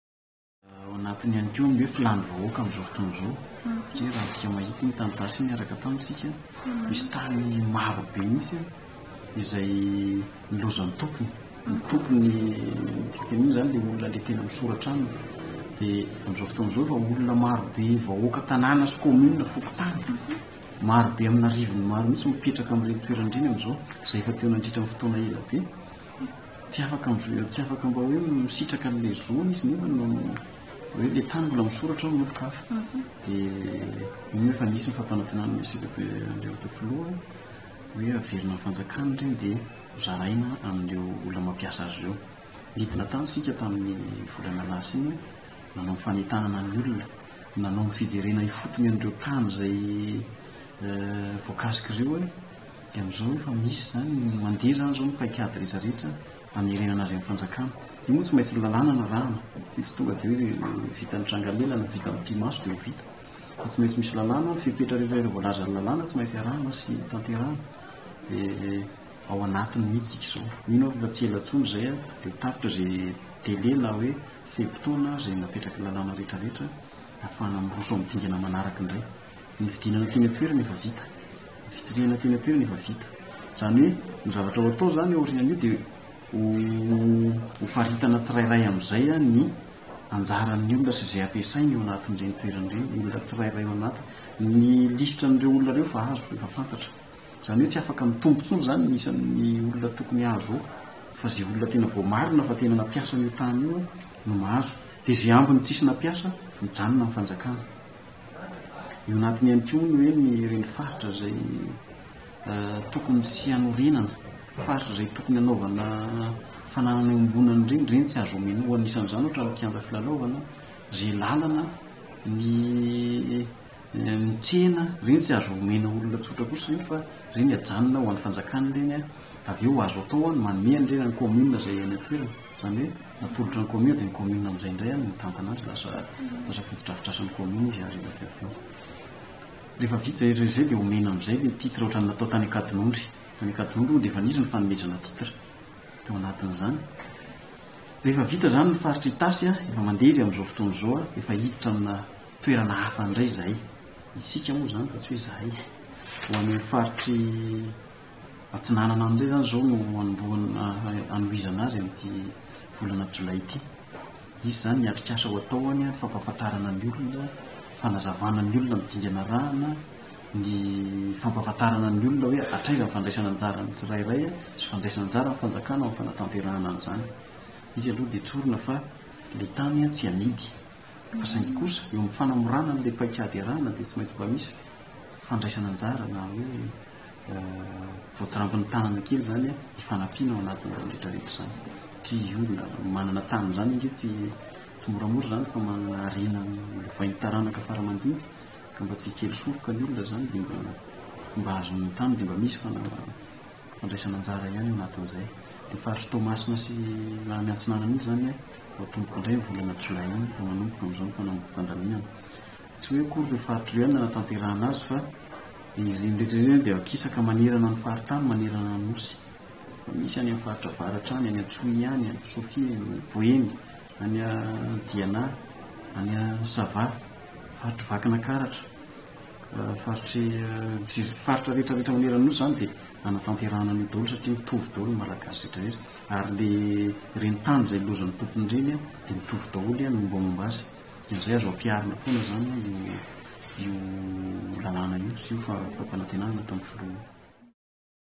Hasimpirenena Rasolomampionona ( Dg Domaine )
journal mada : FANANAN-TANY: Efa miroso amin’ny fanomezana “titre” ireo nanamainty molaly.